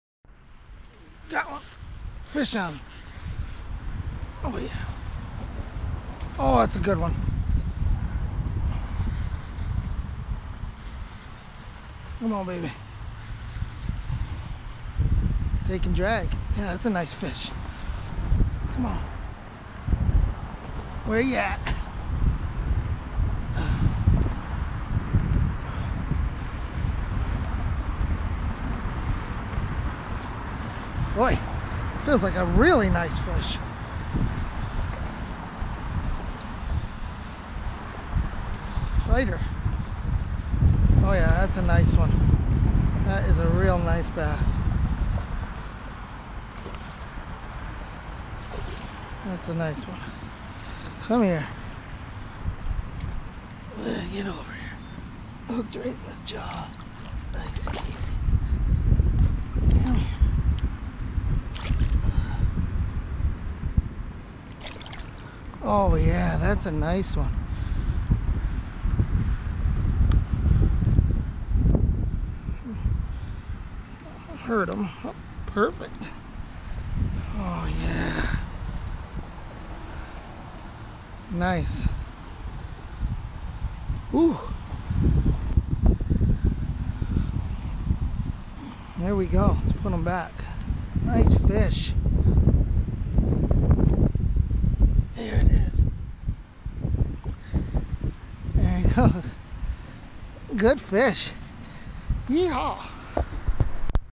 Texas Urban Bass Fishing: 5-12-18 (Fish 1)
Me catching a Large Mouth Bass while fishing an urban pond in the North DWF Metroplex on 5/12/18.